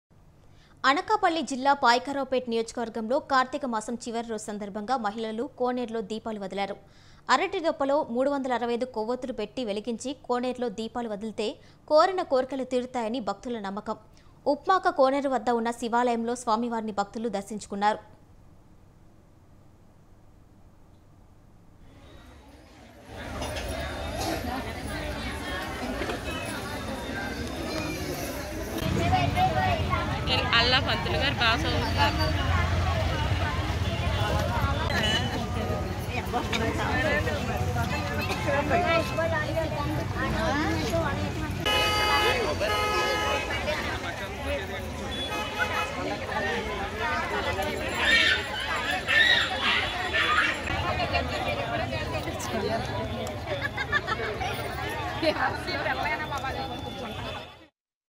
పాయకరావుపేట కోనేరులో దీపాలను వదిలిన మహిళలు | Swatantra TV
Women leaving lamps in Payakaraopet Koneru